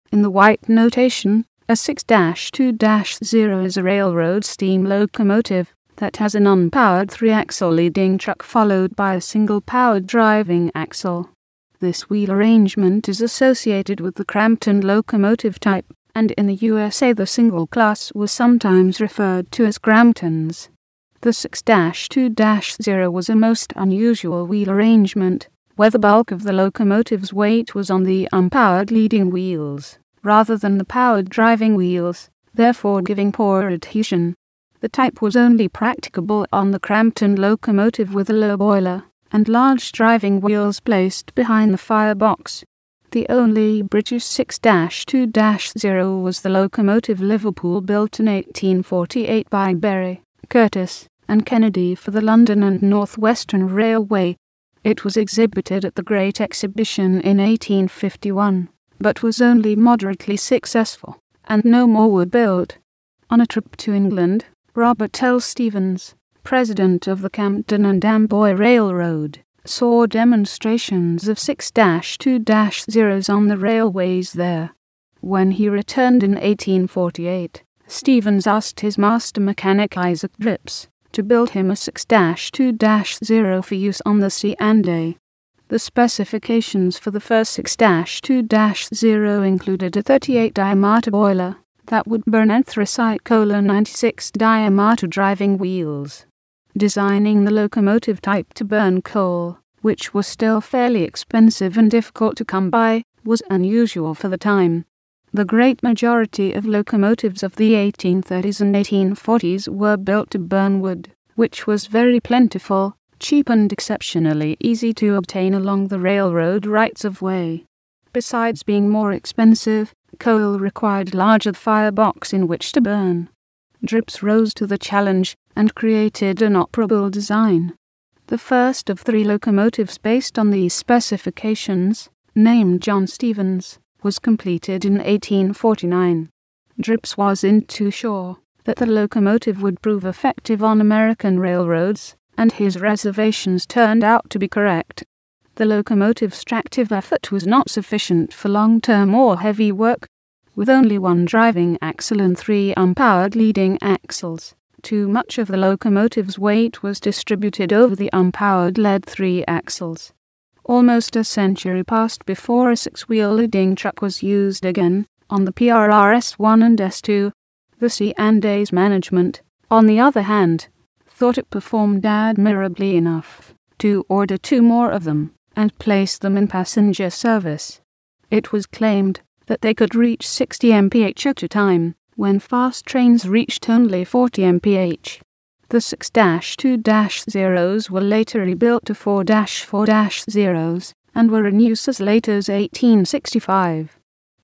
6-2-0
In the Whyte notation, a 6-2-0 is a railroad steam locomotive that has an unpowered three-axle leading truck followed by a single powered driving axle. This wheel arrangement is associated with the Crampton locomotive type, and in the USA the single class were sometimes referred to as Cramptons.

The 6-2-0 was a most unusual wheel arrangement, where the bulk of the locomotive's weight was on the unpowered leading wheels rather than the powered driving wheels, therefore giving poor adhesion. The type was only practicable on the Crampton locomotive with a low boiler and large driving wheels placed behind the firebox.

The only British 6-2-0 was the locomotive Liverpool built in 1848 by Bury, Curtis, and Kennedy for the London and North Western Railway. It was exhibited at The Great Exhibition in 1851 but was only moderately successful and no more were built.

On a trip to England, Robert L. Stevens, president of the Camden and Amboy railroad, saw demonstrations of 6-2-0s on the railways there. When he returned in 1848, Stevens asked his master mechanic Isaac Dripps to build him a 6-2-0 for use on the C&A. The specifications for the first 6-2-0 included a 38' diameter boiler that would burn anthracite coal and 96' diameter driving wheels.

Designing the locomotive type to burn coal, which was still fairly expensive and difficult to come by, was unusual for the time. The great majority of locomotives of the 1830s and 1840s were built to burn wood, which was very plentiful, cheap and exceptionally easy to obtain along the railroad rights of way. Besides being more expensive, coal required a larger firebox in which to burn. Dripps rose to the challenge and created an operable design.

The first of three locomotives based on these specifications, named John Stevens, was completed in 1849. Dripps wasn't too sure that the locomotive would prove effective on American railroads, and his reservations turned out to be correct. The locomotive's tractive effort was not sufficient for long term or heavy work. With only one driving axle and three unpowered leading axles, too much of the locomotive's weight was distributed over the unpowered lead three axles. Almost a century passed before a six-wheel leading truck was used again, on the PRR S1 and S2.

The C&A's management, on the other hand, thought it performed admirably enough to order two more of them and place them in passenger service. It was claimed that they could reach 60 m.p.h. at a time when fast trains reached only 40 m.p.h. The 6-2-0s were later rebuilt to 4-4-0s and were in use as late as 1865.

licensed with CC Attribution-ShareAlike 3.0. This video is licensed with CC Attribution-Share-Alike 3.0 In order to adapt this content it is required to comply with the license terms.